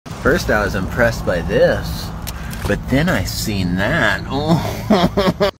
Why the Mississippi River is so fun to fish
The Mississippi River is like a box of chocolates. 🍫
You never know what you’re gunna get. 🤷‍♂️

We spent a chilly morning searching for walleye. The bite was tough at first, but slowly picked up once the sun came out.
Randy captained the boat like a champ - fighting the wind and putting us on the fish.

After we caught a couple dozen gravel lizards, we accidentally hooked into this prehistoric beast. He put up a good fight and was safely released.

OUR FISHING SETUP
➜ Flourocarbon Line 12#
➜ Powerpro Braided Line 15#
➜ Rapala Folding Bump Board
➜ Frabill Landing Net
➜ St. Croix Spinning Rod - ​​
➜ Pflueger President Reel
➜ ⅜ oz Jig
➜ Bass Assassin Walleye Turbo Shad